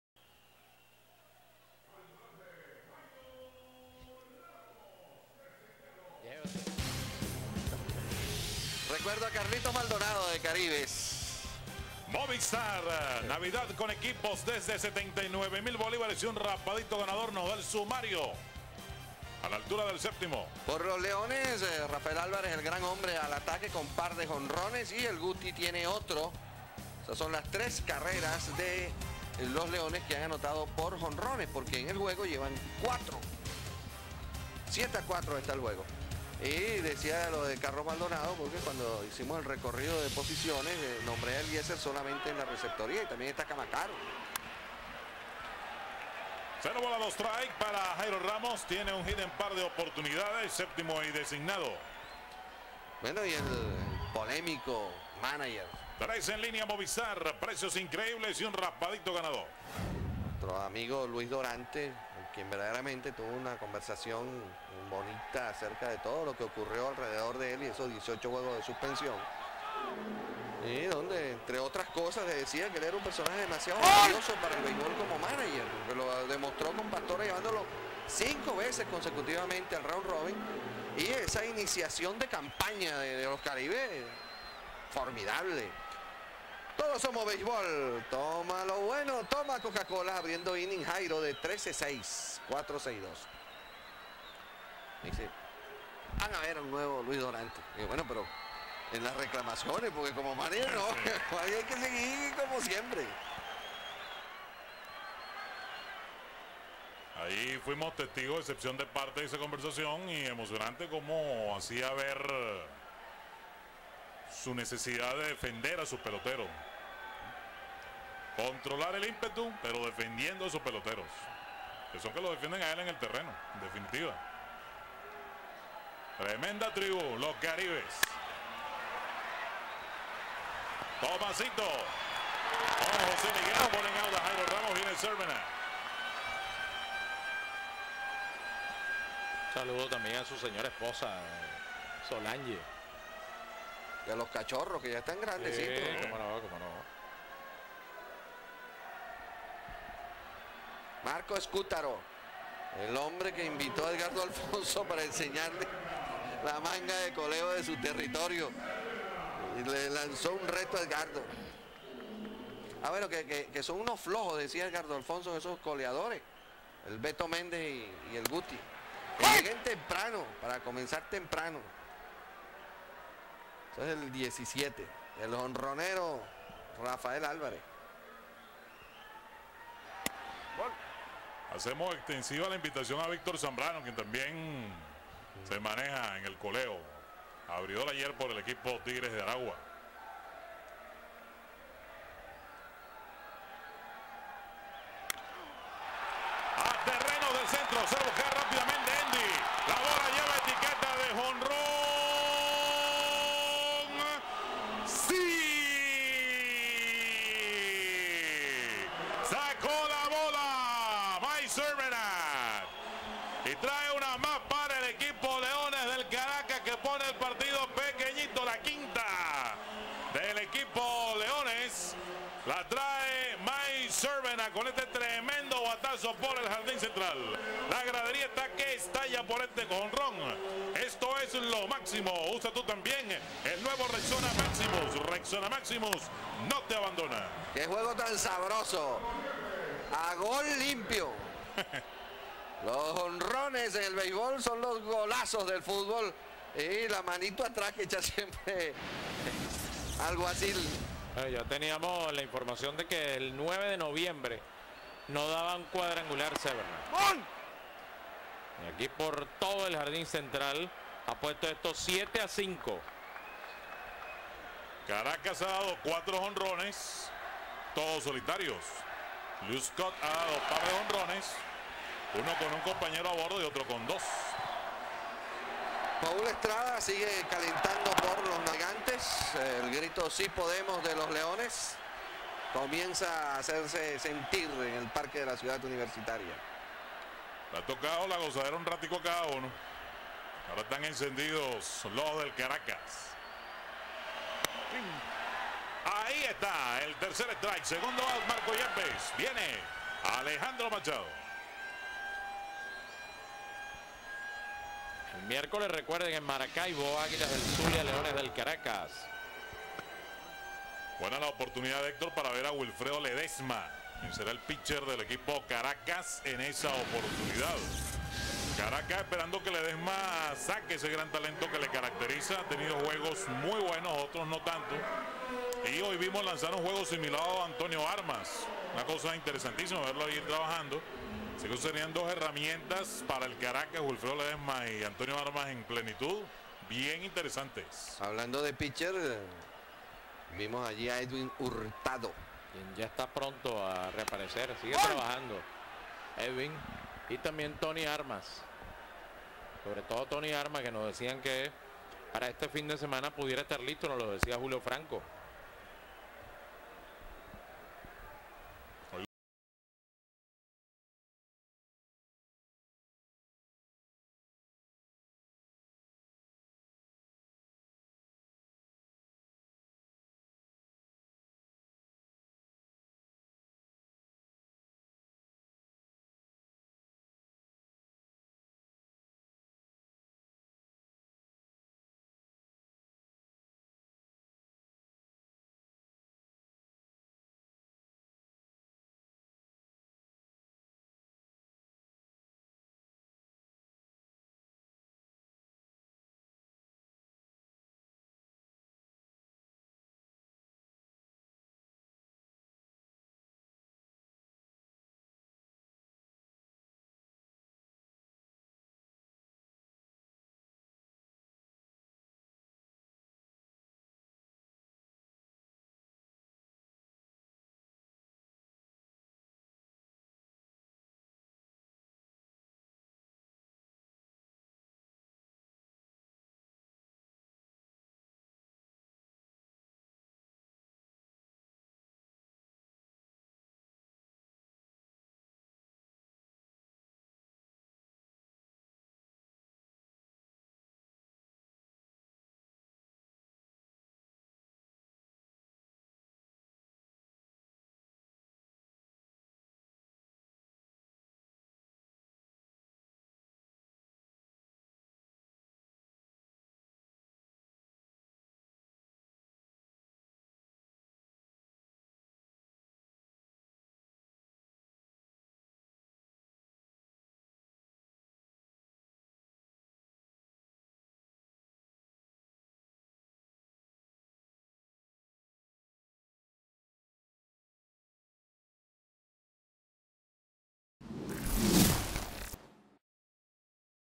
Jonron de Mike Cervenak - Leones del Caracas vs Navegantes del Magallanes Temp. 2005-2006 LVBP
Cuadrangular de Mike Cervenak en el encuentro entre los Navegantes del Magallanes y los Leones del Caracas el 12 de diciembre de 2005 en el estadio Universitario de Caracas, Temporada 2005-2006 de la Liga Venezolana de Beisbol Profesional.

Cortesía: Venevision